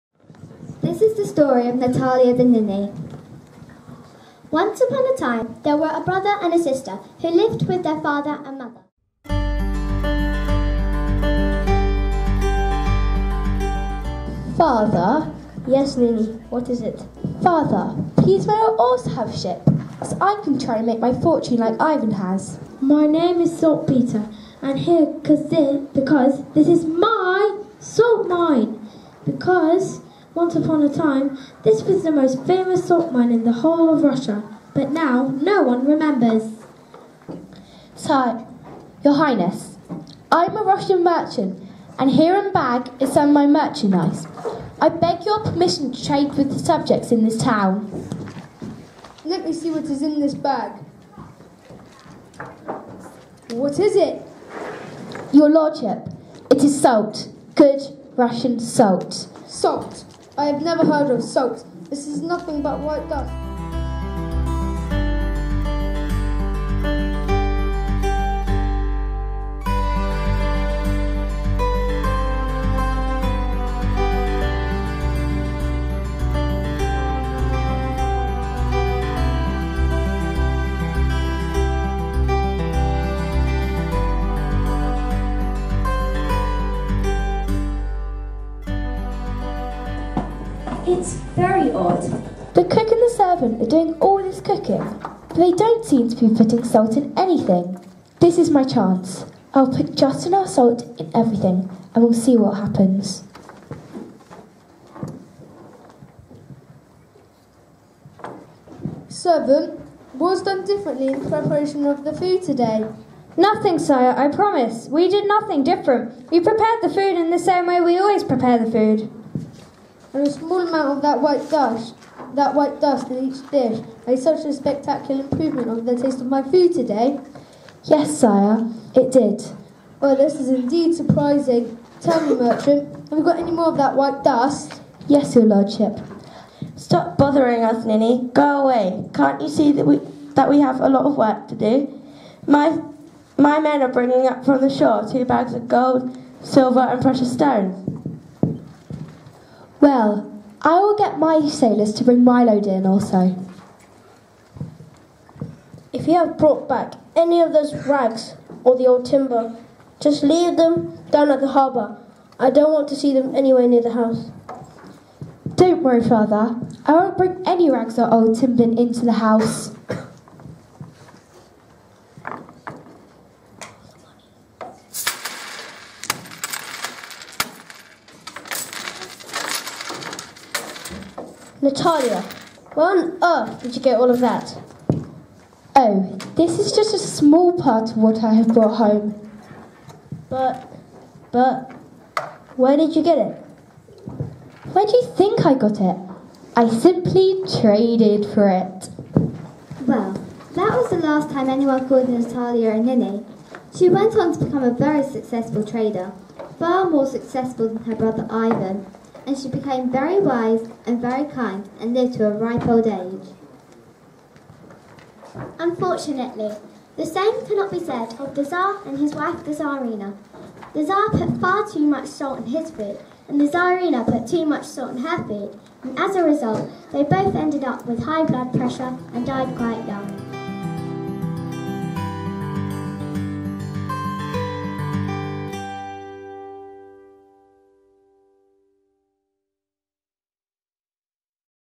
Natalya the Ninny (Bromet Primary) 2018 - the highlights
Highlights from Bromet Primary's performances of Natalya the Ninny, a Russian fairytale, in July 2018.